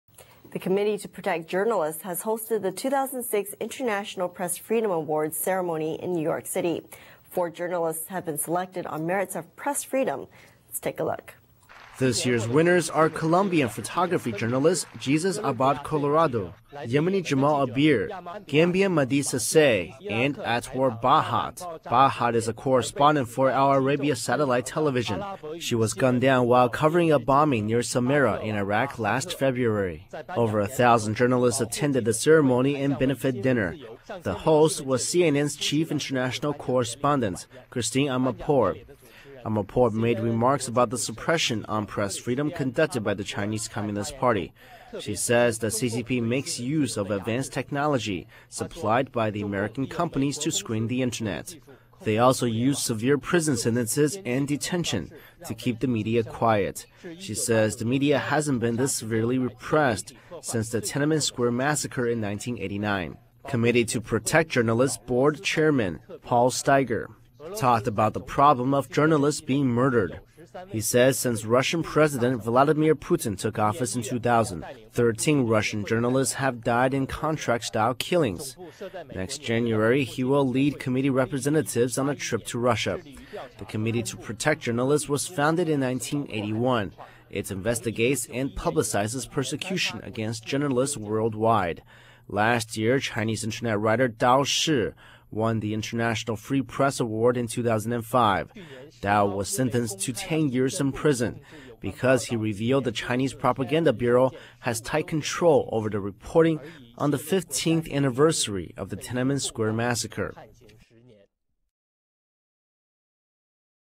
International Press Freedom Awards Held in New York
Online  Download
CHAN: The Committee to Protect Journalists has hosted the 2006 International Press Freedom Awards ceremony in New York City. Four journalists have been selected on merits of press freedom.

STORY: This year's winners are Colombian photography journalist Jesus Abad Colorado, Yemeni Jamal Amer, Gambian Madi Ceesay and Atwar Bahjat.

Bahjat is a correspondent for Al-Arabiya satellite television. He was gunned down while covering a bombing near Samarra in Iraq last February.

During the ceremony Hodding Carter the third received a lifetime achievement award for protecting press freedom.

Over a thousand journalists attended the ceremony and benefit dinner. The host was CNN's Chief International Correspondent Christiane Amanpour.

Amanpour made remarks about the suppression on Press freedom conducted by Chinese Communist Party. She says the CCP makes use of advanced technology supplied by the American companies to screen the Internet.

They also use severe prison sentences and detention to keep the media quiet. She says the media hasn't been this severely repressed since the Tiananmen Square Massacre in 1989.

Committee to Protect Journalists board chairman Paul Steiger, talked about the problem of journalists being murdered.

He says since Russian President Vladimir Putin took office in 2000, 13 Russian Journalists have died in contract-style killings.

Next January he will lead Committee representatives on a trip to Russia.

The Committee to Protect Journalists was founded in 1981. It investigates and publicizes persecution against journalists worldwide.

Last year, Chinese Internet writer Tao Shi won the International Press Freedom Award. In 2005.

Tao was sentenced 10 years in prison because he revealed the Chinese Propaganda Bureau has tight control over reporting on the 15th anniversary of the Tiananmen Square Massacre.